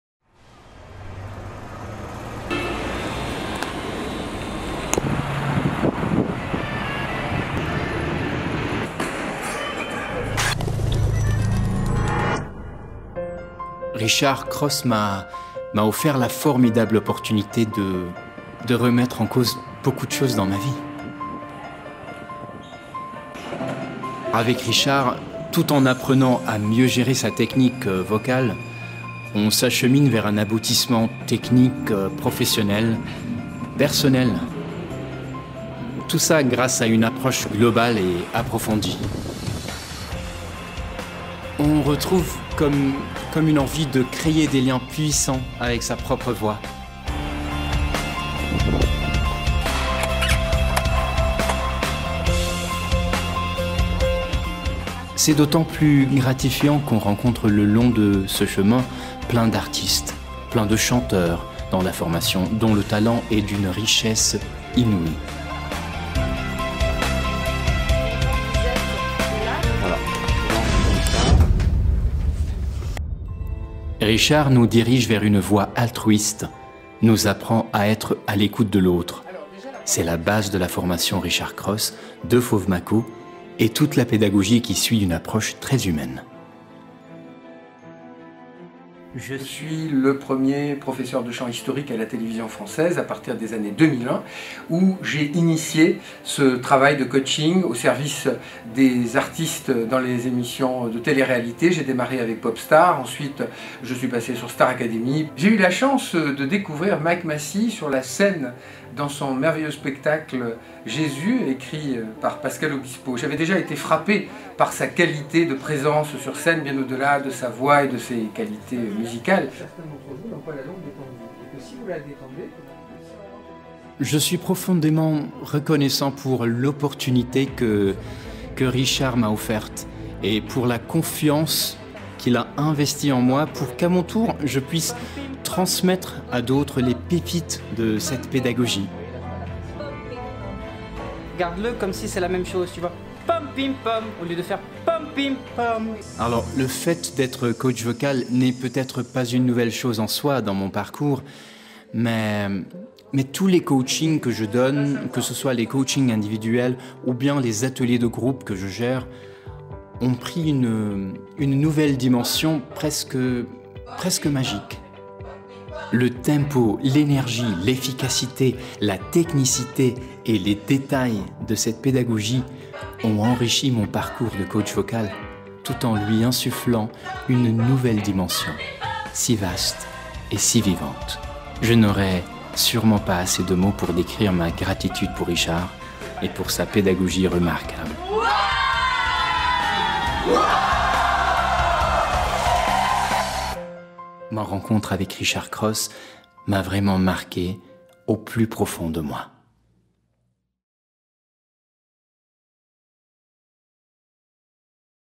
Mike Massy - Hommage À Richard Cross
Concept, Text & Voice Over : Mike Massy
Camera Operator : Matthieu Khairallah
Editing : Charbel Sammour
Post Production : Sary Asmar
Proof Reading : Nami Moukheiber

Richard Cross m’a offert la formidable opportunité de remettre en cause beaucoup de choses dans ma vie.
Avec Richard, tout en apprenant à mieux gérer sa technique vocale,
on s’achemine vers un aboutissement technique, professionnel et personnel, grâce à une approche globale et approfondie.
On retrouve comme une envie de créer des liens puissants avec sa propre voix.
C’est d’autant plus gratifiant qu’on rencontre le long de ce chemin plein d’artistes, plein de chanteurs dans la formation, dont le talent est d’une richesse inouie !
Richard nous dirige vers une voie altruiste, nous apprend à être à l’écoute de l’autre…
C’est la base de la formation Richard Cross, de Fovmaco? et toute la pédagogie qui suit une approche très humaine.
Je suis profondément reconnaissant pour l’opportunité que Richard Cross m’a offerte, et pour la confiance qu’il a investie en moi, pour qu’à mon tour je puisse transmettre à d’autres les pépites de cette pédagogie.
Le fait d’être coach vocal n’est peut-être pas une nouvelle chose en soi dans mon parcours, mais tous les coachings que je donne, que ce soit les coachings invidviduels, ou bien les ateliers de groupe que je gère, ont pris une nouvelle dimension, presque magique.
Le tempo, l’énergie, l’efficacité, la technicité et les details de cette pédagogie ont enrichi mon parcours de coach vocal, tout en lui insufflant une nouvelle dimension, si vaste et si vivante  !
je n’aurai sûrement pas assez de mots pour décrire ma gratitude pour Richard et pour sa pédagogie remarquable.
Ma rencontre avec Richard Cross m’a vraiment marqué au plus profond de moi !